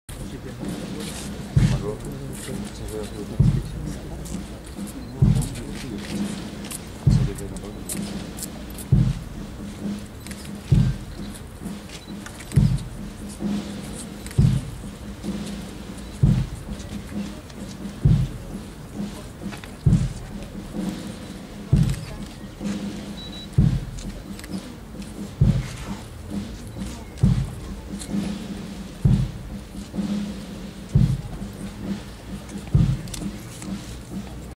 День Победы (23)
Возложение венков к памятнику павшим советским воинам на Ольшанском кладбище. 9 мая 2010 года. г.Прага